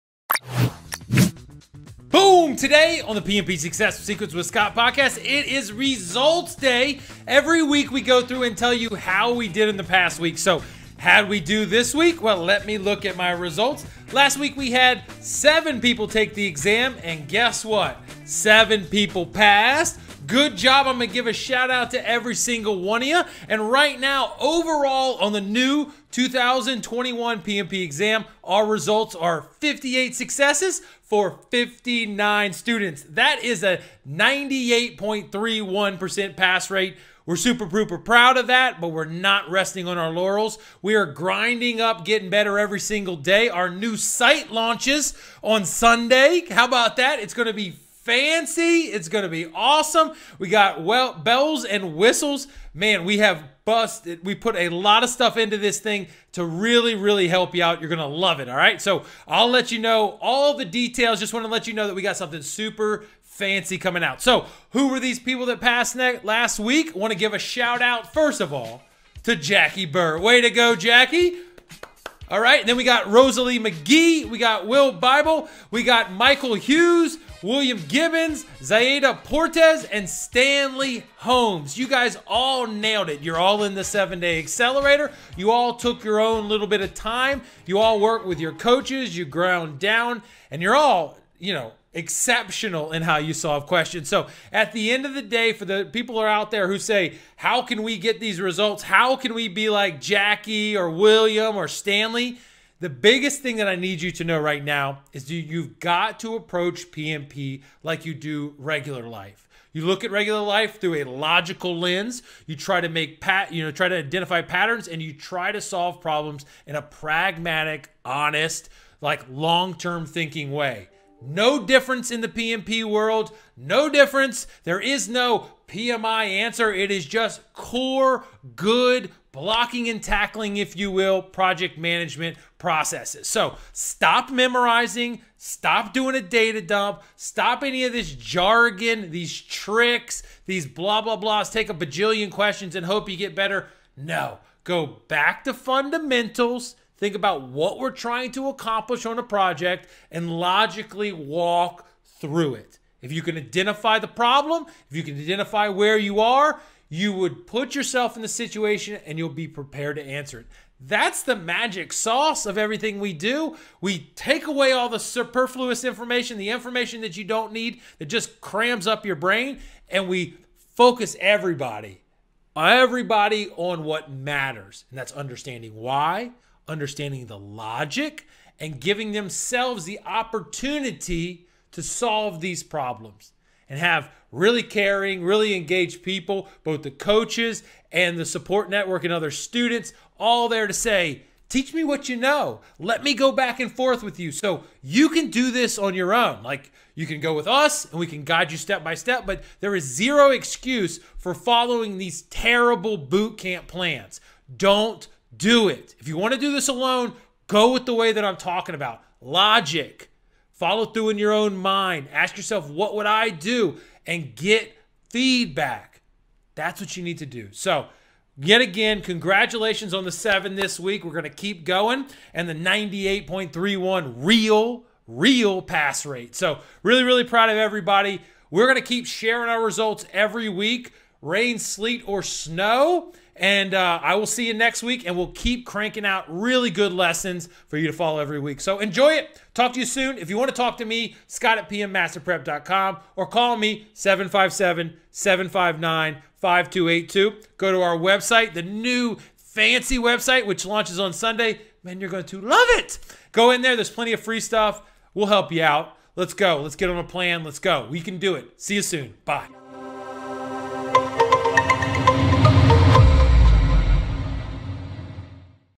PMP 2021 EXAM Weekly Success Summary | April 27- May 4, 2021
Here is our PMP Success Summary for the NEW PMP 2021 Exam from Last Week
We were 7 for 7 PMP Exam Success (PERFECT!!!)

Overall Results: 58 for 59 (98,31% 1st time Success Rate)

➤ Stop Struggling and Wasting your Valuable Time!
➤ Join my 7-day Accelerator Program!
➤ My team and I will personally guide you to your PMP
➤ No limits on time or support